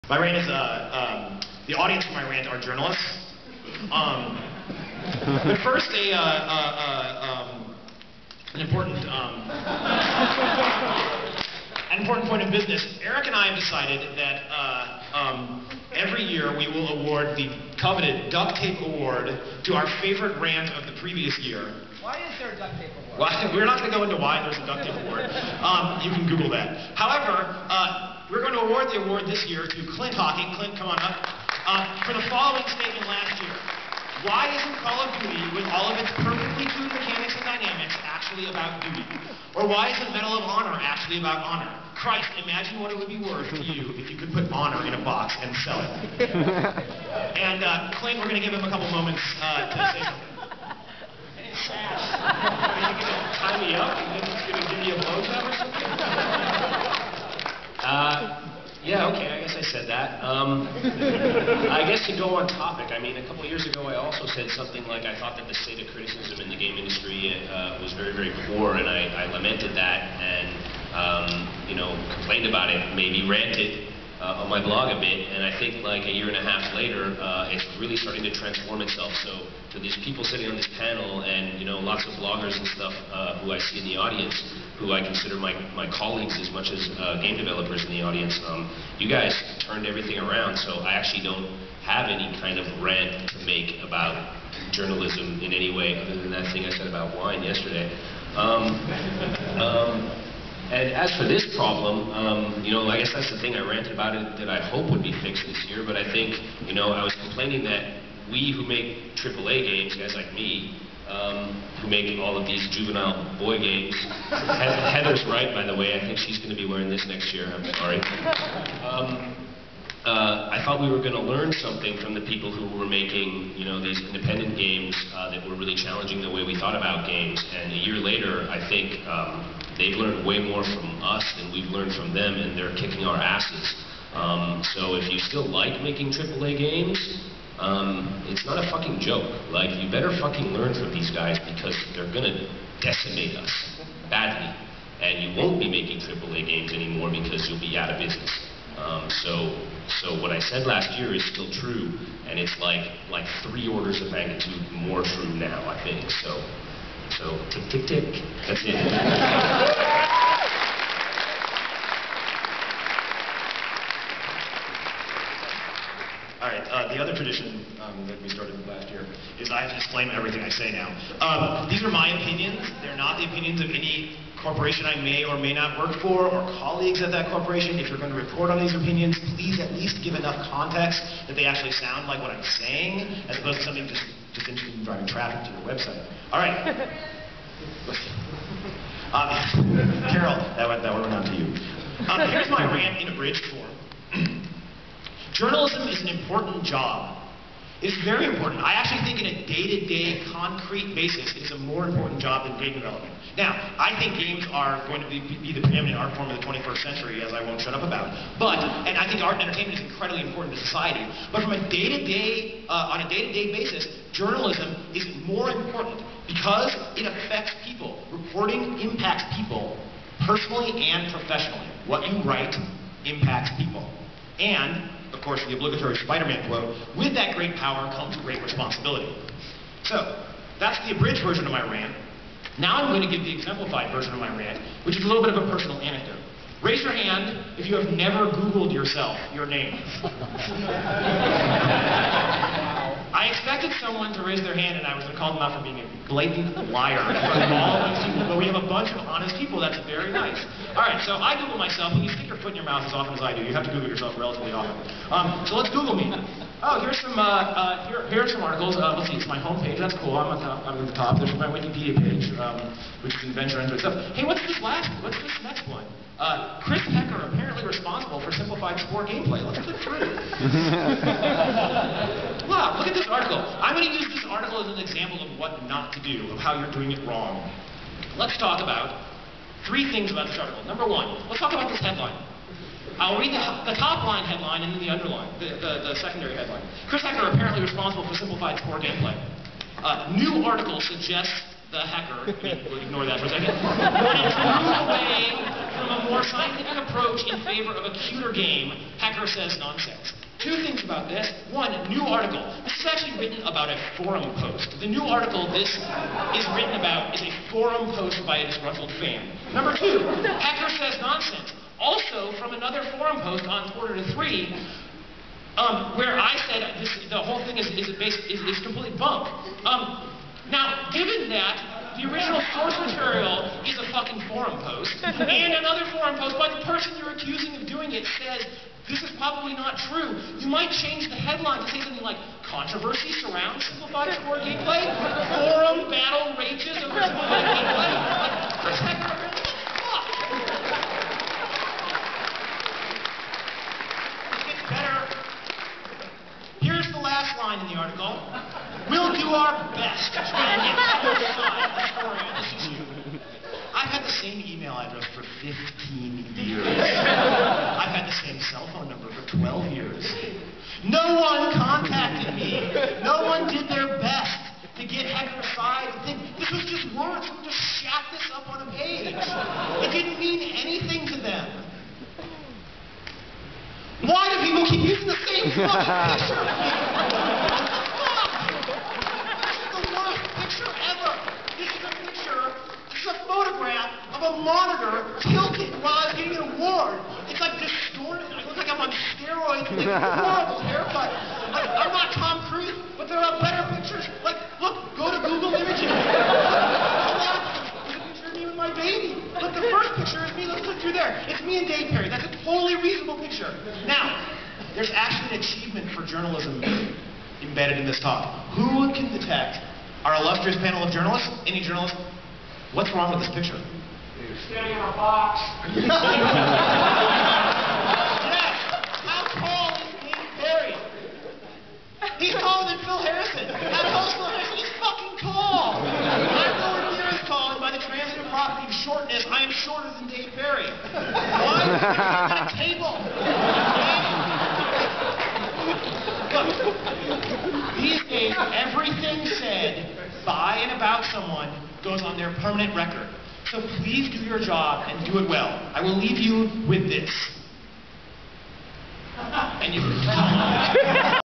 Chris Hecker's GDC Rant at GDC 09
Part of a multi-guest panel of game journalists ranting about the industry.